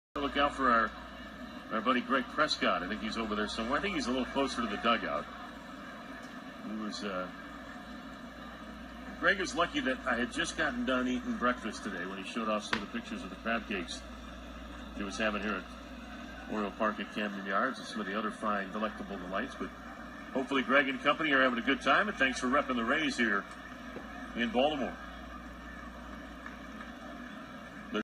David Wills R.I.P. 1965-2023, From One Radio Call to Another
David Wills, the longtime voice of the Tampa Bay @Rays died early this morning at the age of 58 years old. For many in the Tampa Bay community, since he announced the team on radio and television starting in 2005, the Rays community cherished his calls through both the 2008 World Series run and through the midst of a pandemic during the shortened 2020 baseball season where the team went to the World Series in that shortened year. The voice of the Rays was also less well known to a national audience, but he was a sweet, gentle, and very kind announcer, who students like me should aspire to be. Professional and pure.

Broadcasters are people too, and so was the case for one of the most iconic voices in Tampa Bay history as baseball commentary has lost yet another legend. The team has decided to cancel their broadcast during the Rays spring training game vs. the Baltimore Orioles who will pay tribute to Wills during their spring training game on March 5 at 1:05 pm.

And gentle was Mr. David Wills and his family was may he rest in peace.

David Wills, head play-by-play announcer for Tampa Bay Rays
1965- March 5, 2023